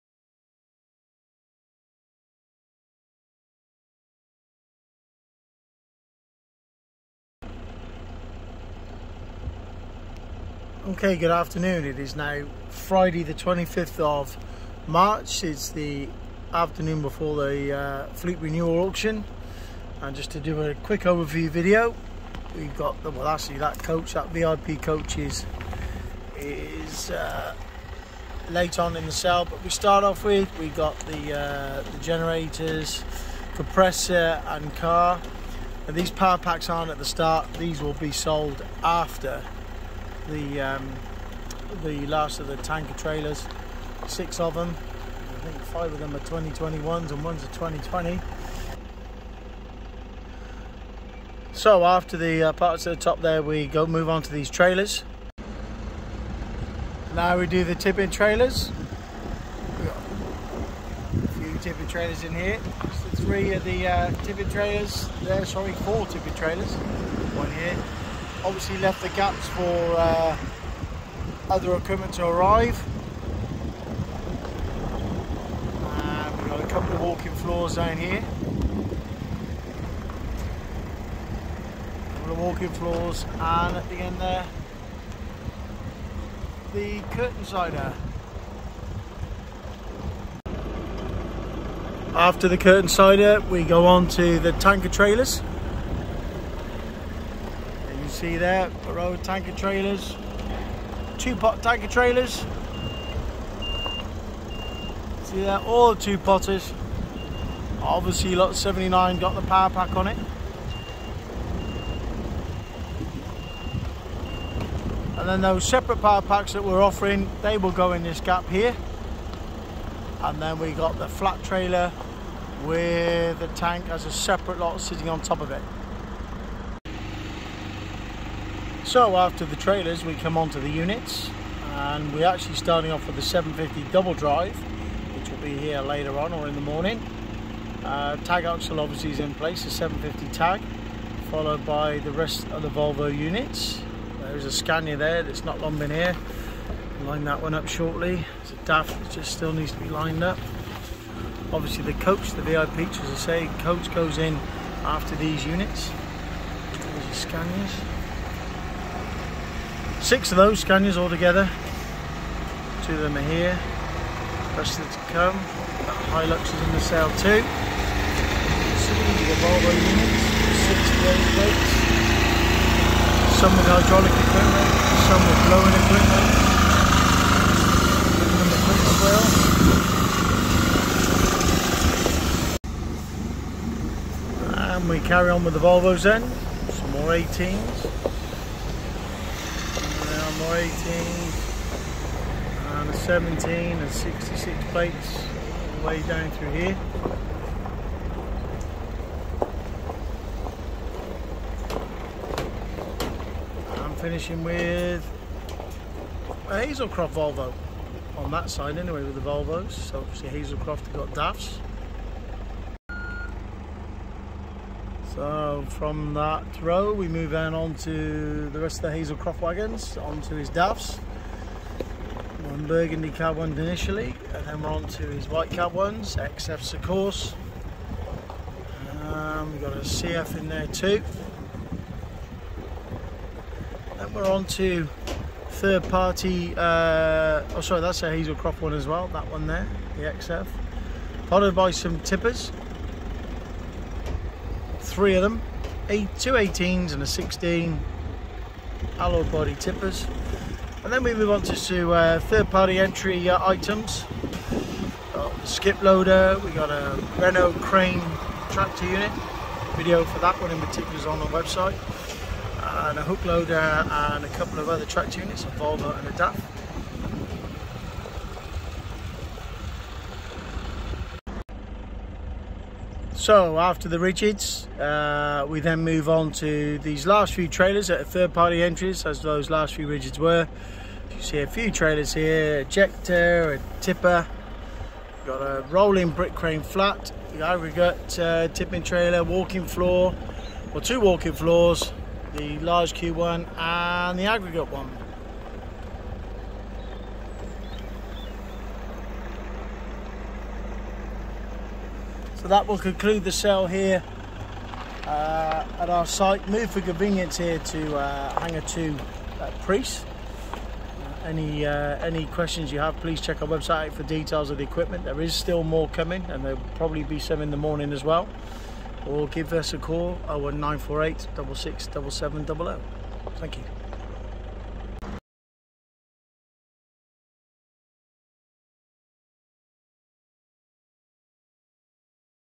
FLEET RENEWAL AUCTION Overview Video
FLEET RENEWAL AUCTION - SATURDAY, MARCH 26TH 2022 09:00 AM GMT

Prees Airfield Hangar 2, A41, Whitchurch, SY13 2JL